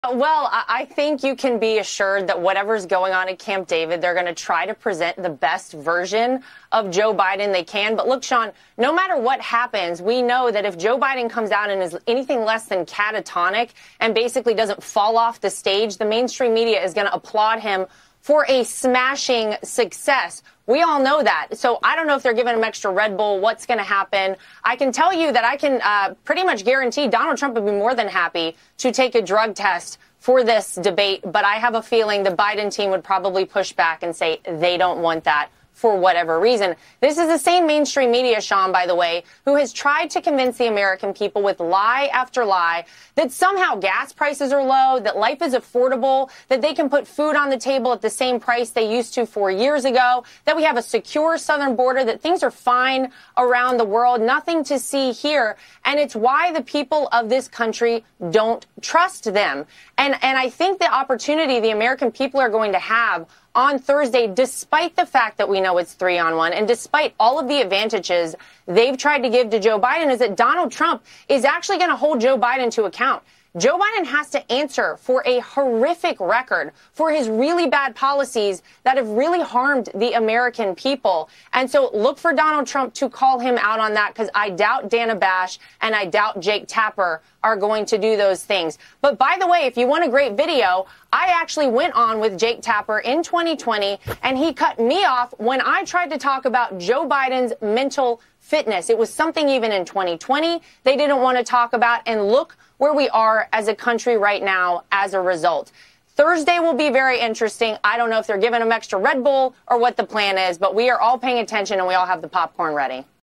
RNC Co-Chair Lara Trump: President Trump will hold Biden accountable during the debate!
RNC Co-Chair Lara Trump: President Trump will hold Biden accountable for his record during the debate!